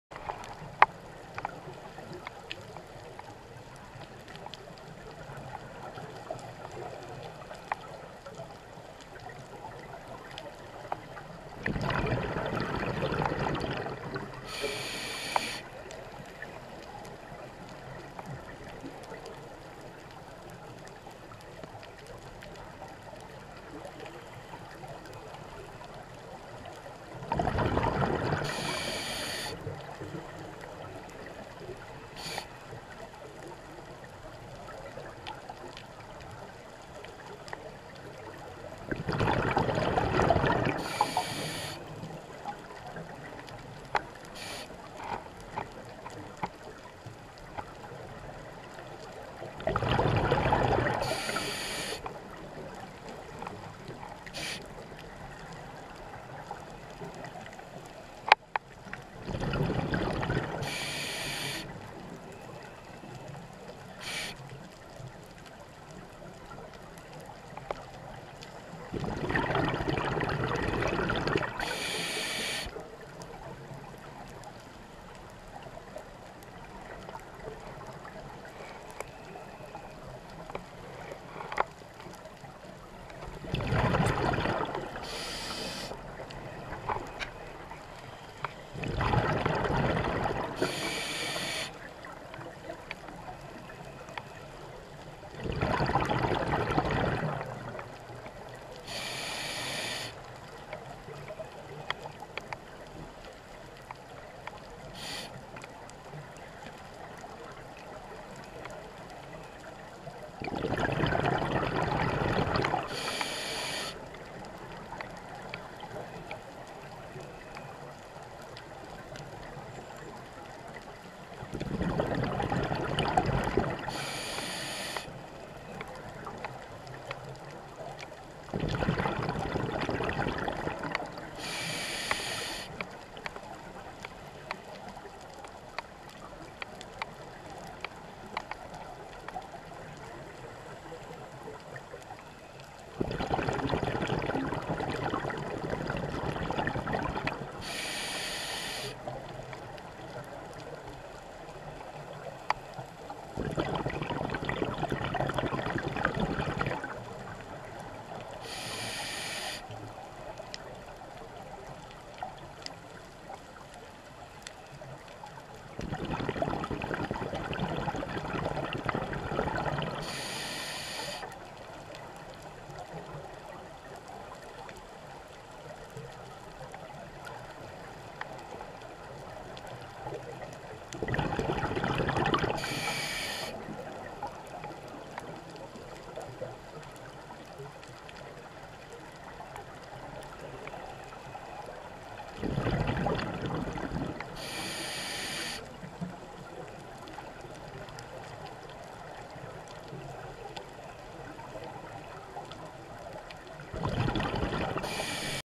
Blue Corner dive site in Palau
Black tip reef sharks and Trigger fish.